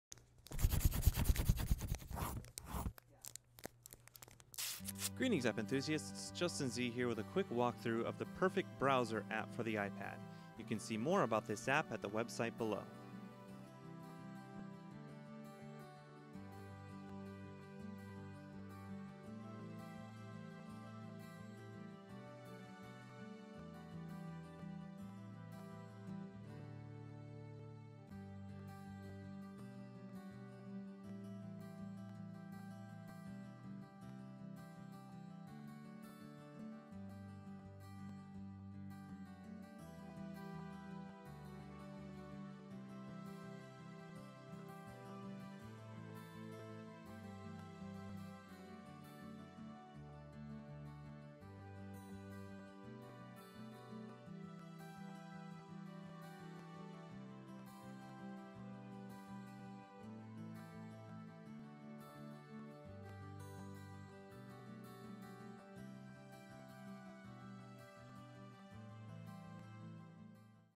Privacy Settings in the Perfect Browser App for the iPad
A quick walkthrough of privacy settings in the Perfect Browser App for the iPad. You can see more about this app